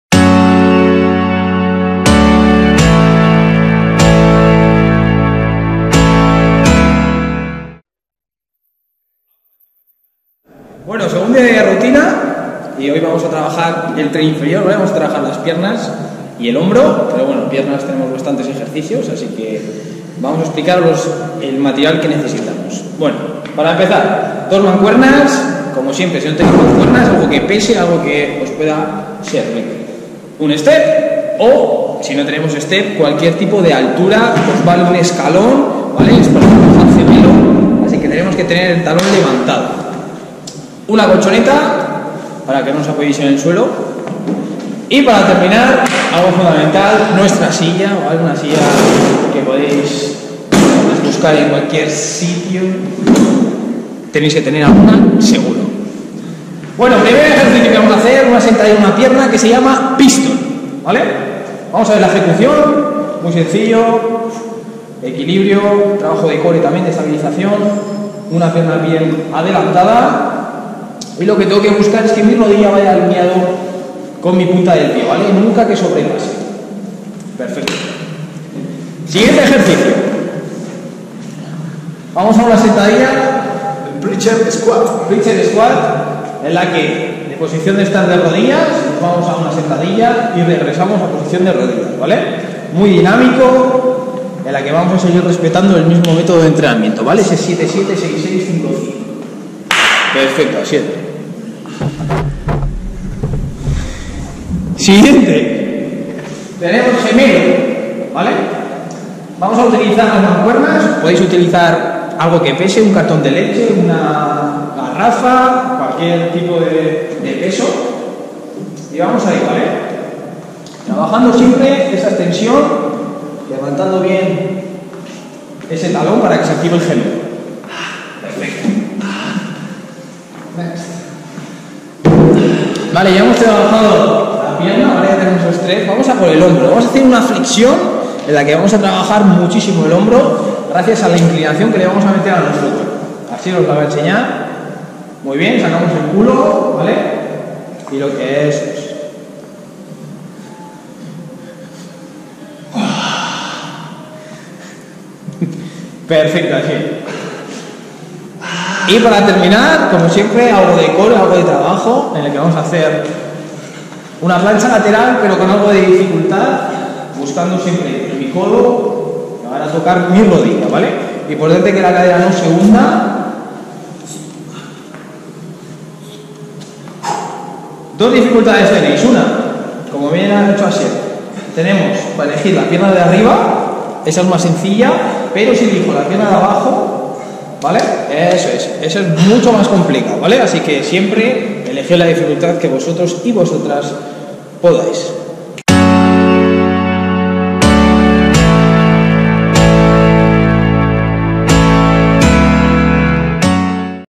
Entrenamiento AR7 - Día2
Rutina basada en un entrenamiento que se llama AR7. Se hacen 3 vueltas de un mismo ejercicio con series descendientes. Primero, 7 series de 7 repeticiones (7x7) con un descanso de 7 segundos entre serie y serie, cuando finalices las 7 series descansas 2 minutos. Y hacemos lo mismo con 6x6 (6” de descanso), descansas 2 minutos y terminas con, 5x5 (5” de descanso).